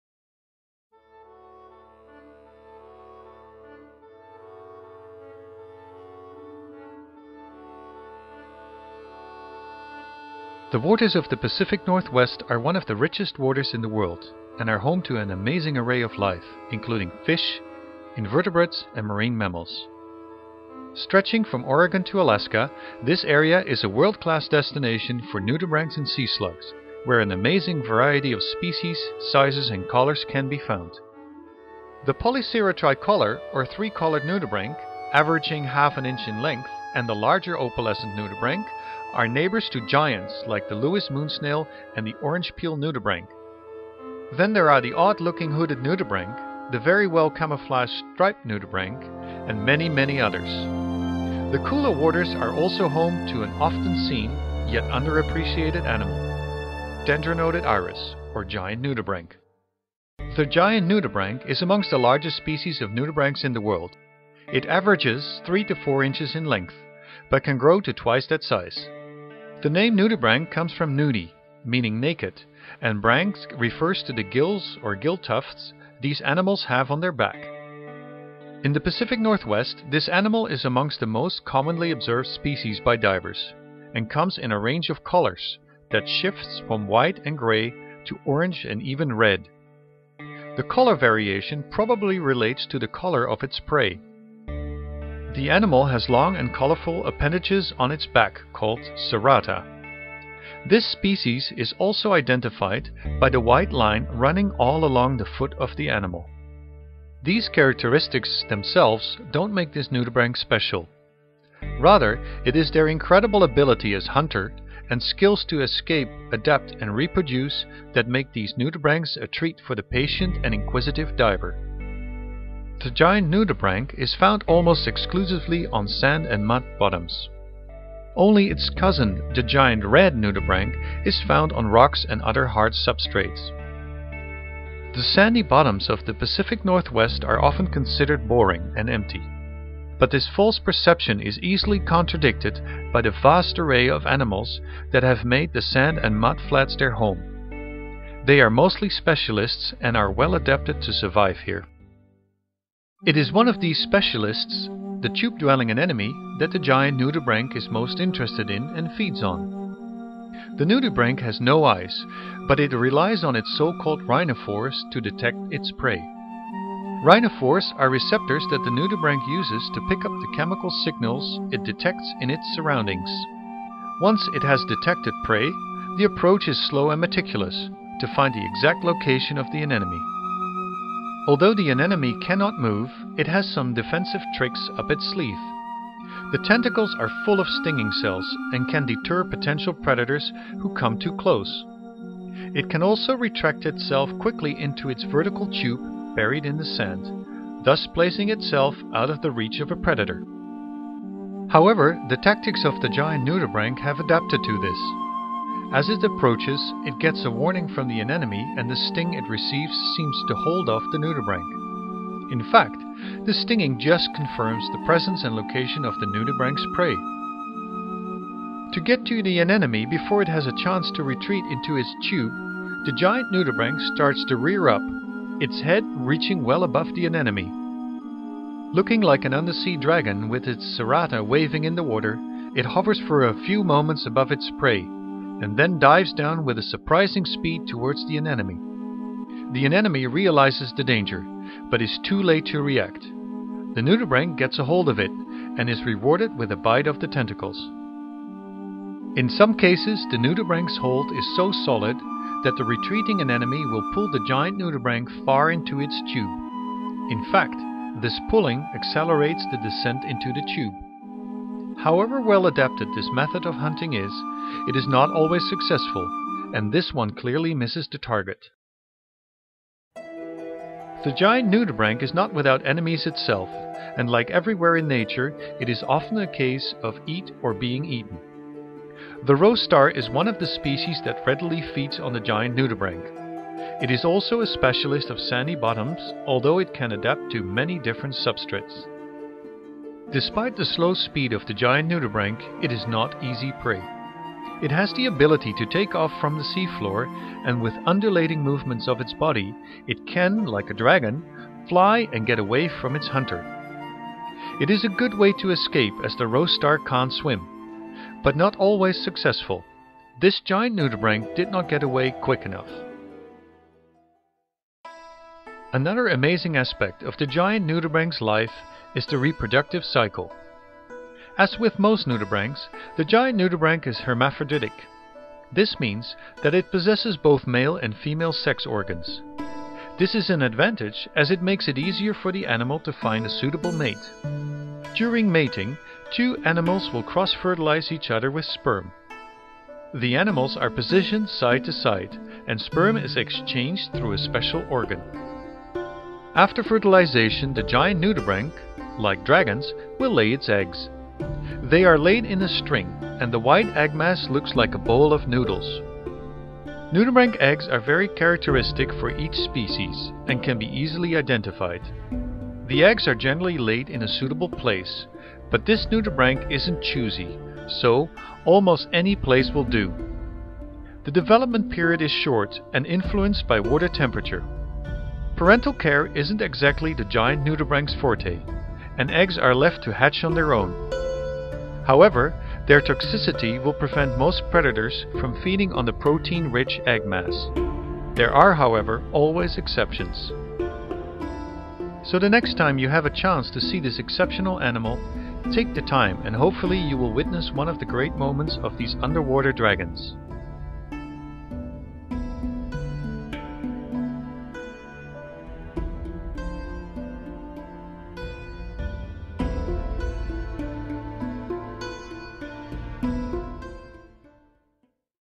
the underwater dragon, lifecycle of the giant nudibranch, 水中ドラゴン、巨大ウミウシのライフサイクル
A short video of the life cycle of the giant nudibranch. Great footage for a natural history story. much of the original footage available in 4K.
巨大なウミウシのライフサイクルの短いビデオ。自然史の物語にぴったりの素晴らしい映像。オリジナルの映像の多くは 4K で利用できます。